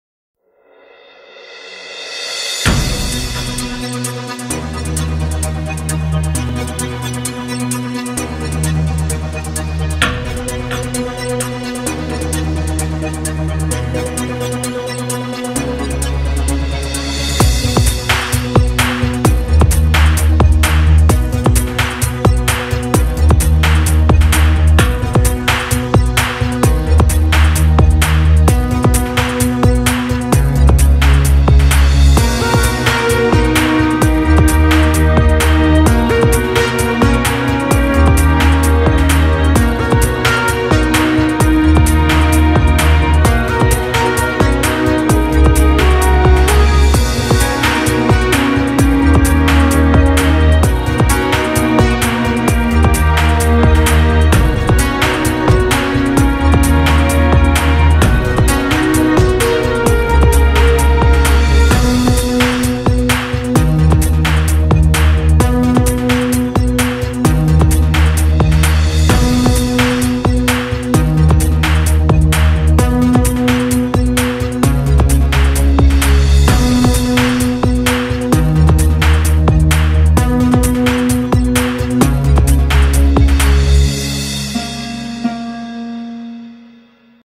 ABB YuMi and 800XA automation systems helping Telefonica in Argentina
ABB YuMi and automation system 800XA is helping Telefonica in Argentina to improve their systems reliability while reducing costs.